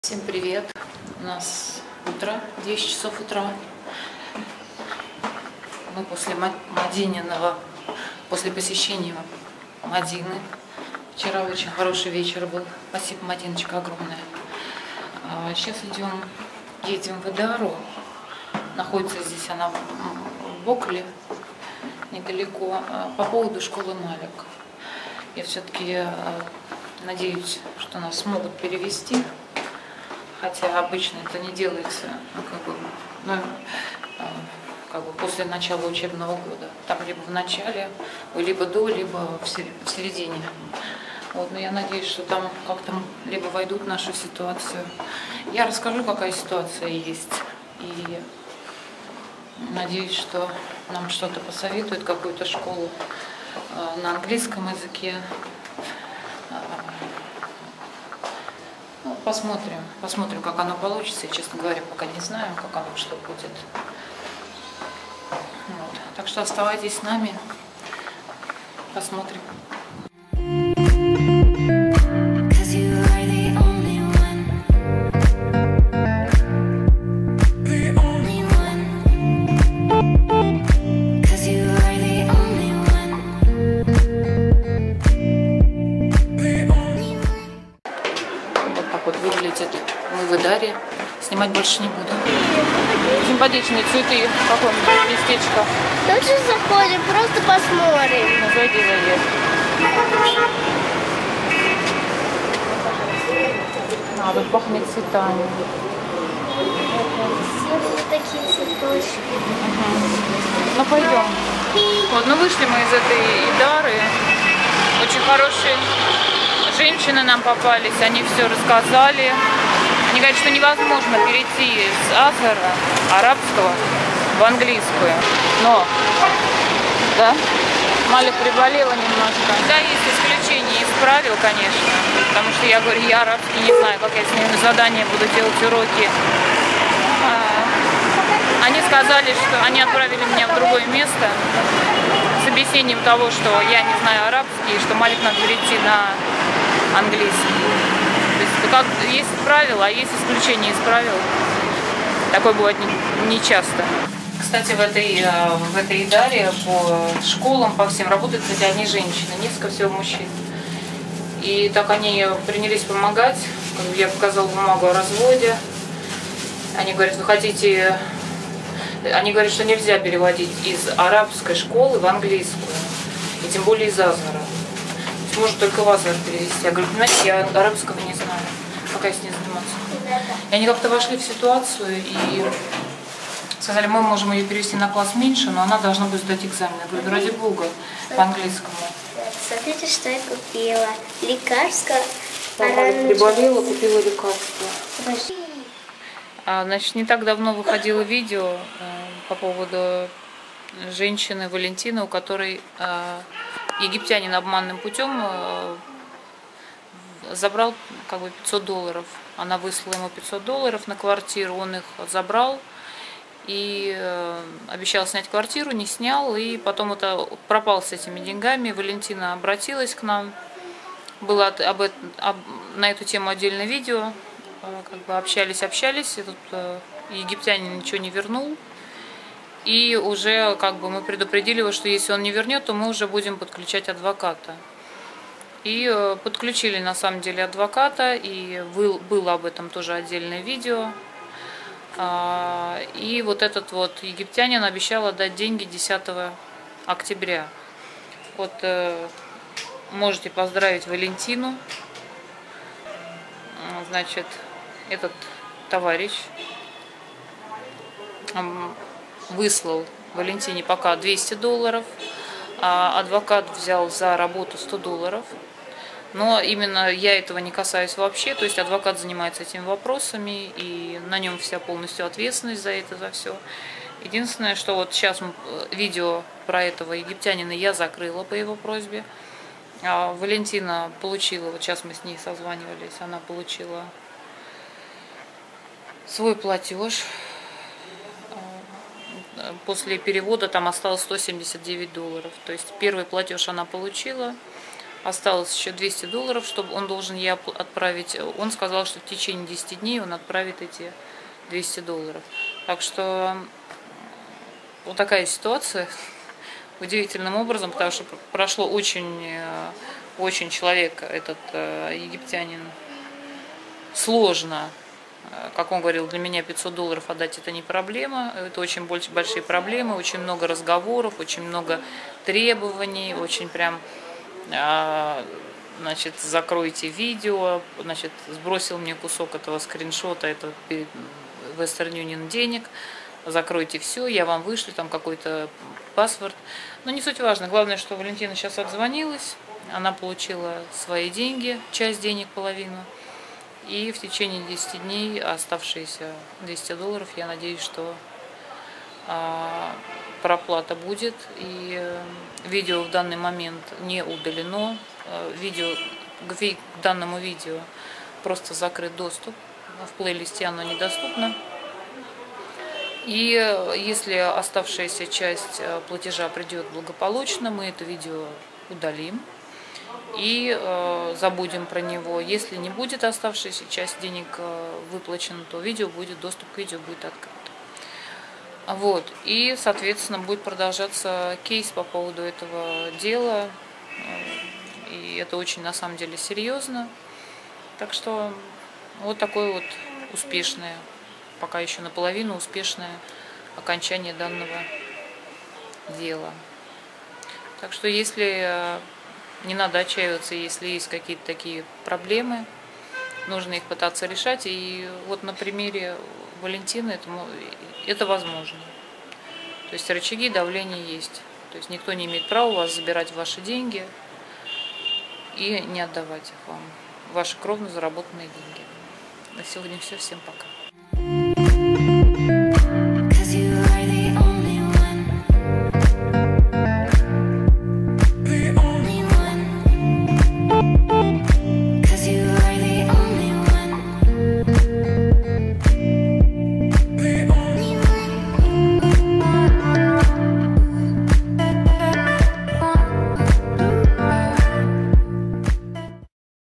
ЕГИПЕТ: ВЕРНУЛИ УКРАДЕННЫЕ ДЕНЬГИ 🤑 ШКОЛА ДОЧЕРИ НОВОСТИ
Как возвращали обманутой египтянином подписчице деньги перейти сразу 03:15 . В этом влоге вы узнаете как удалось обманутой египтянином подписчице вернуть чатсь ее денег. Также новости по поводу перевода моей дочери в другую школу.
Комментарии и комментаторы носящие оскорбительный характер и ругательства блокируются без предупреждения.